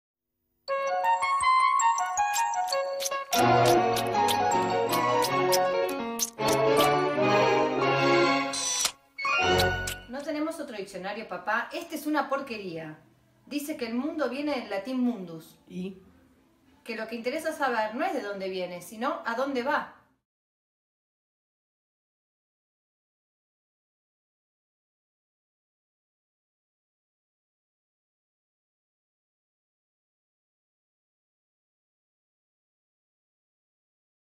La Pequeña filosofía de Mafalda #13
La Pequeña filosofía de Mafalda. Todos leemos a Mafalda, una pastilla de felicidad en estos días de cuarantena.
Agustina Martínez - Imanías, Argentina - lee Mafalda.